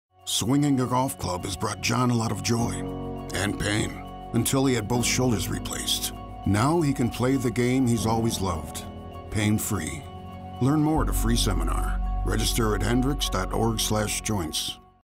Swing with Pain-free Shoulders
Swinging a golf club has brought John of Avon a lot of joy — and pain. When his discomfort became too much, John returned to the doctors he trusted at Hendricks Regional Health for his two shoulder replacements. Thanks to the expert care he received from his surgeon and physical therapist, he can now play the game he’s always loved. Pain free. Talk with orthopedic surgeon, Dr. Chad Waits, at a free seminar.